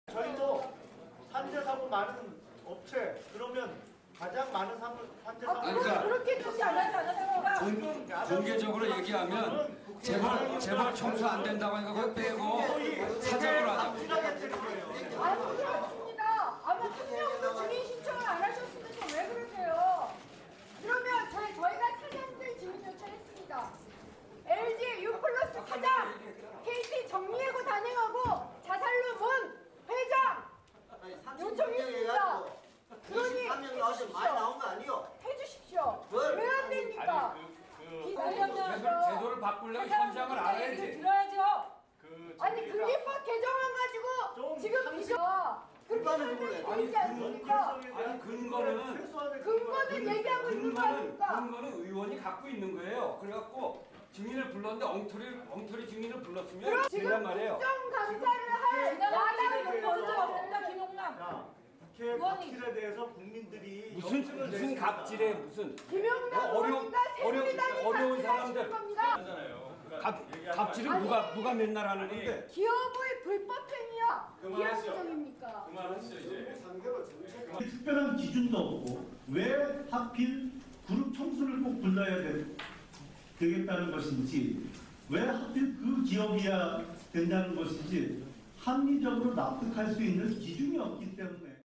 ‘증인 싸움’ 환노위 또 파행
정몽구 현대자동차그룹 회장, 이재용 삼성그룹 부회장, 신동빈 롯데그룹 회장 등 재벌총수들을 국정감사 증인으로 채택할 지를 두고 여야 간 힘겨루기가 계속되면서 국회 환경노동위원회 국감 파행이 이틀째 계속되고 있다. 8일 환노위 여야 간사들은 오전 10시 고용노동부에 대한 국감에 앞서 증인채택 협의를 가졌지만 이견을 좁히지 못해 11시 40분께 뒤늦게 시작, 40분간 입씨름만 이어가다 결국 정회됐다. /신현종 기자

[국감현장] '기업인 증인 싸움' 환노위 이틀째 파행